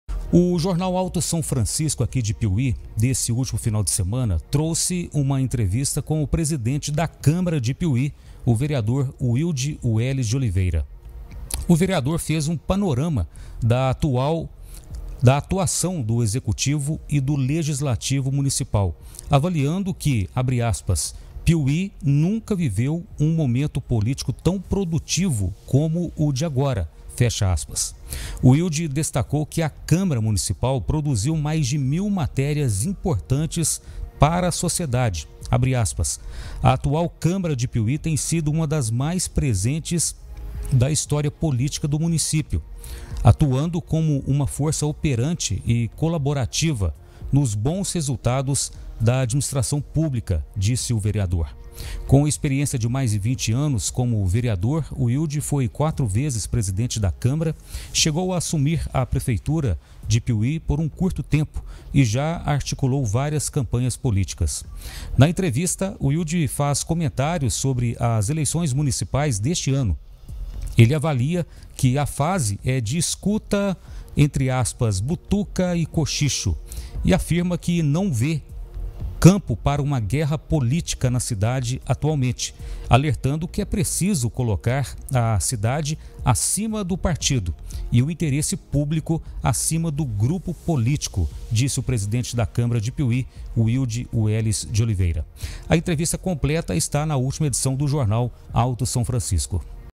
PRESIDENTE WILDE WÉLLIS DIZ QUE ATUAÇÃO DA CÂMARA DE PIUMHI É UMA DAS MAIS MARCANTES DA HISTÓRIA
| PIUMHI |

O vereador fez um panorama da atuação do executivo e legislativo, avaliando que “Piumhi nunca viveu um momento político tão produtivo como agora”. Veja mais: